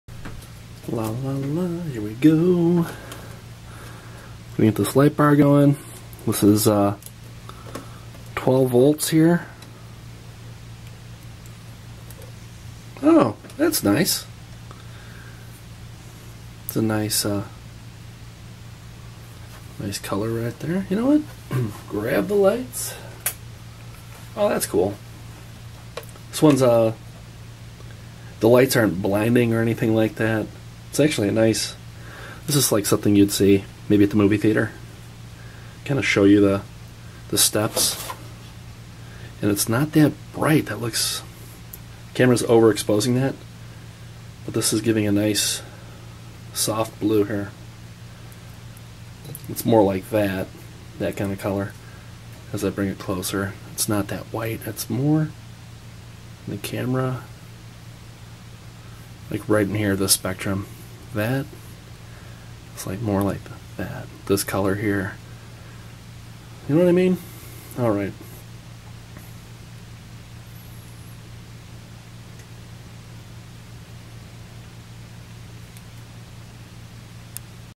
G26099 - Blue Light Tube 12" Long 12VDC @ 0.125A
Blue light tube is 12" long x 0.44" dia. and has a light blue tint to it. Inside the plastic tube is a PC Board containing 18 bright blue T1 3mm LEDs. The LEDs light up a bright blue when 12VDC @ 0.125Amp is applied. Yellow lead is (+) and black lead is (-).